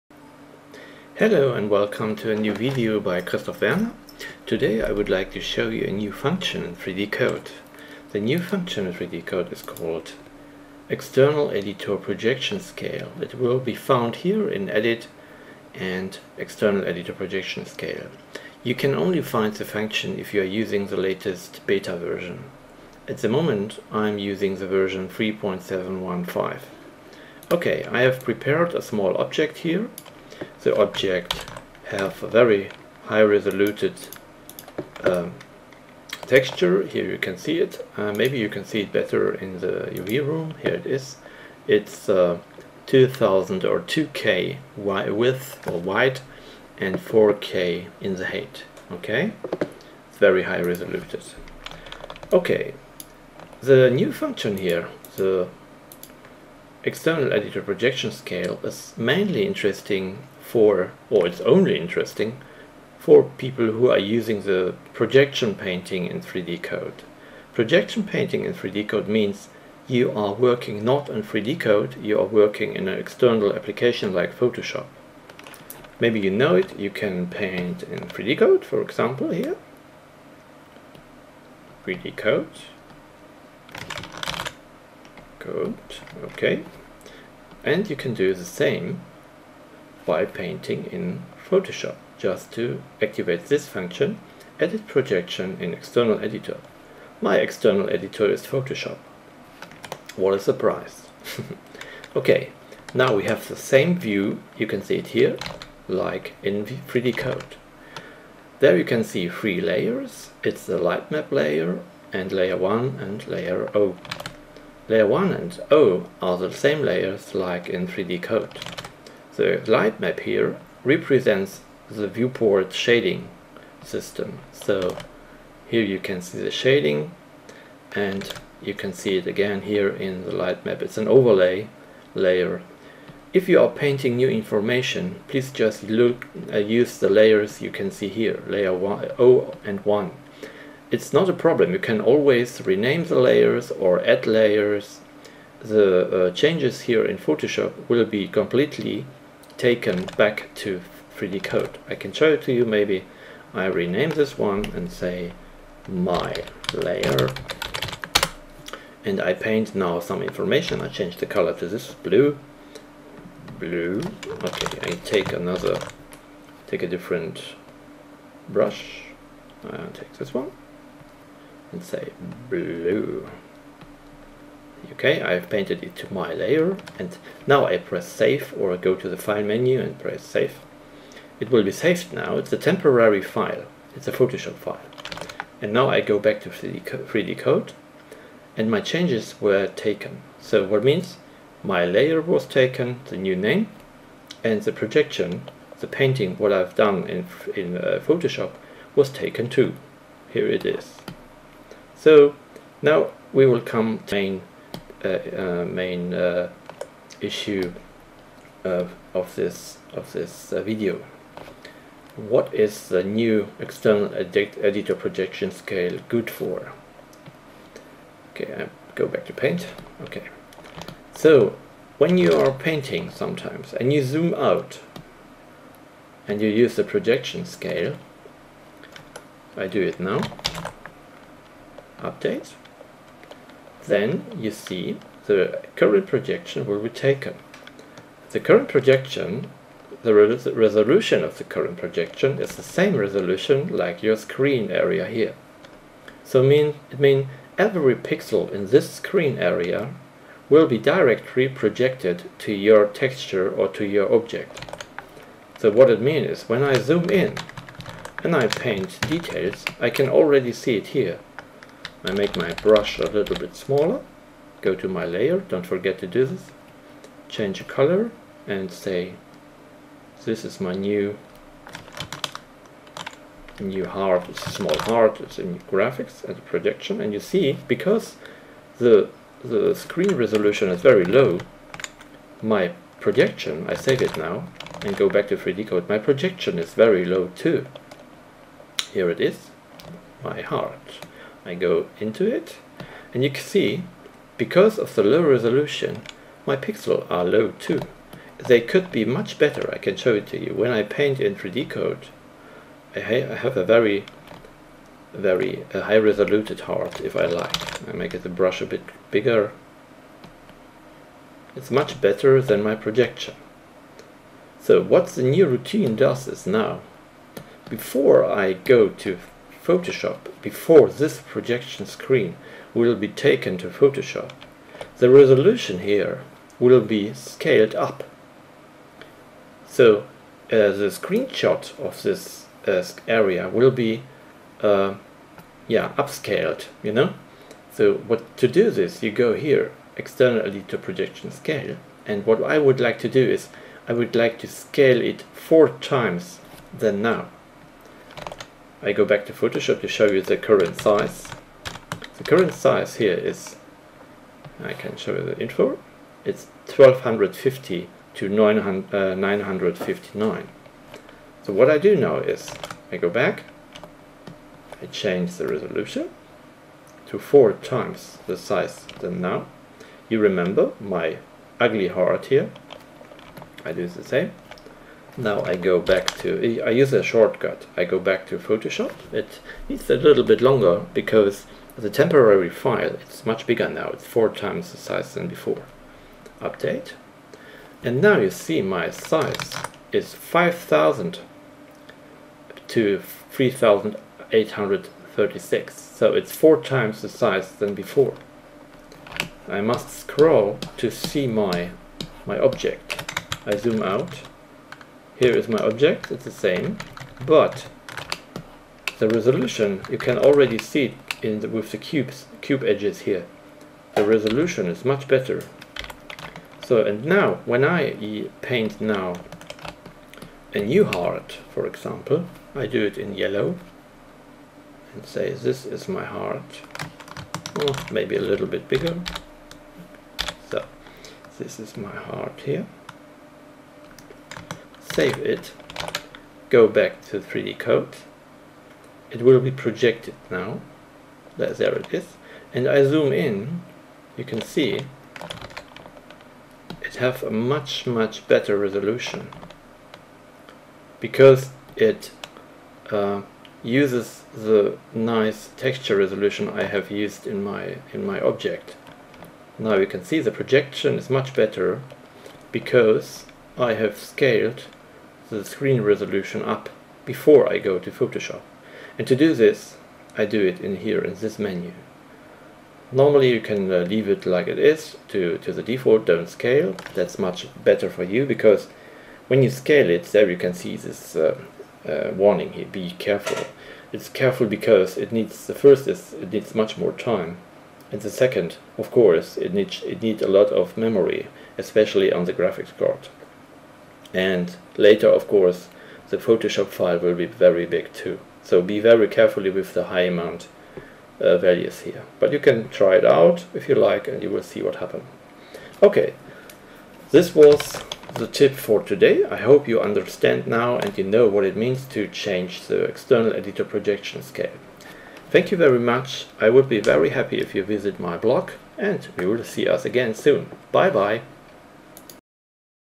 Understanding the external editor projection scaling in 3D Coat
3D Coat Tutorial. You will learn and understand how to use the external editor projection scaling function.

Please consider to to make a donation on my blog Thank you very much for watching.